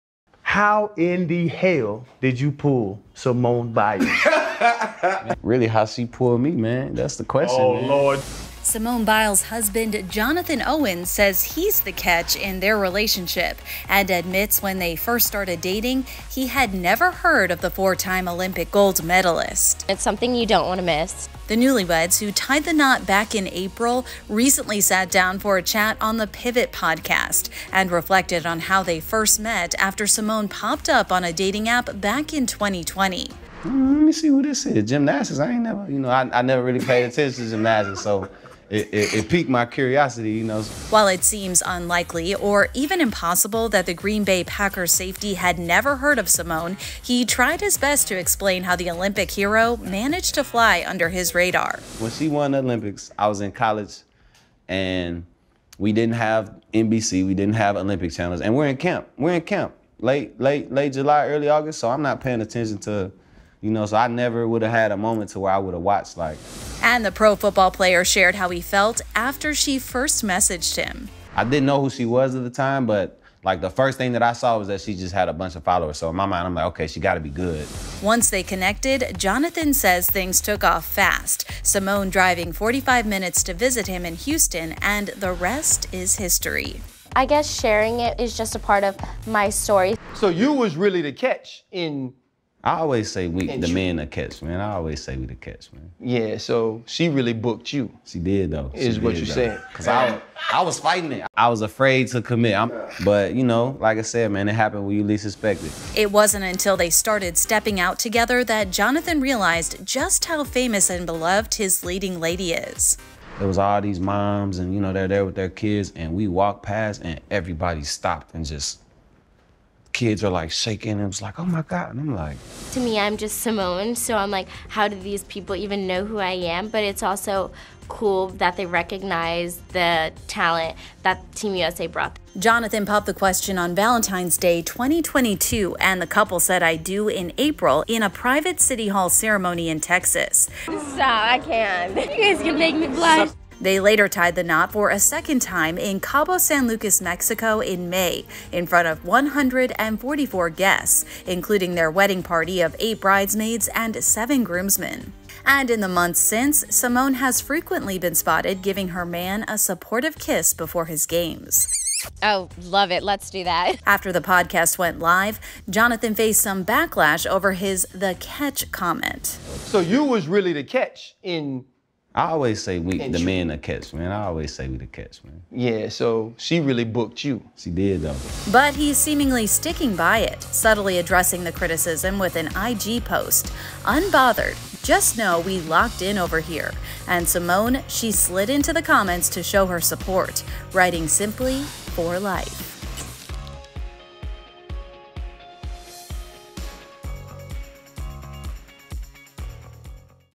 Simone Biles' Husband Jonathan Owens Says He's the 'Catch' Over Her
Simone Biles and her husband, Green Bay Packers safety Jonathan Owens, stopped by ‘The Pivot’ podcast and opened up about their relationship.